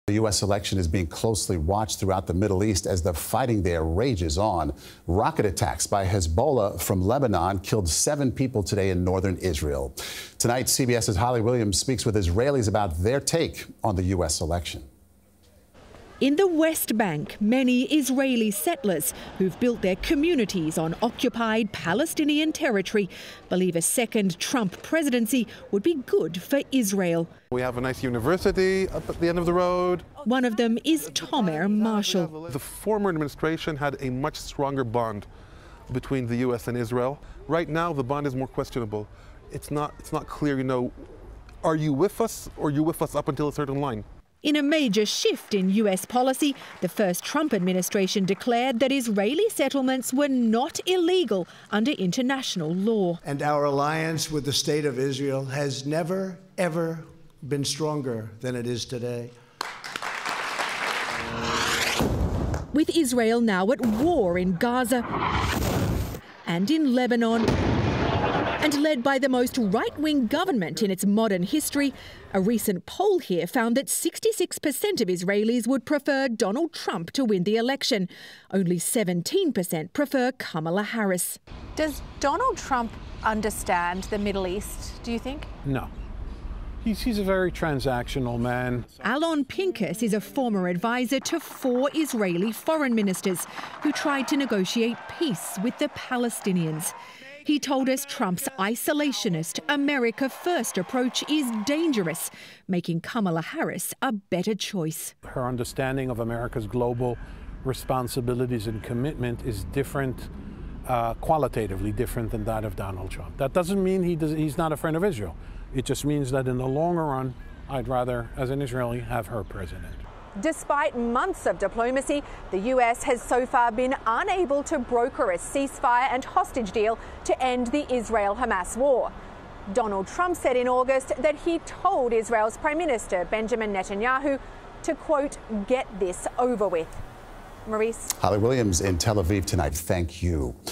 How Israelis see the U.S. election
With Israel at war in Gaza and in Lebanon, and led by the most right-wing government in its modern history, the Middle East is closely watching the results of the U.S. election, which could determine U.S. foreign policy in the region. Holly Williams spoke to Israelis about their perspective of the November election.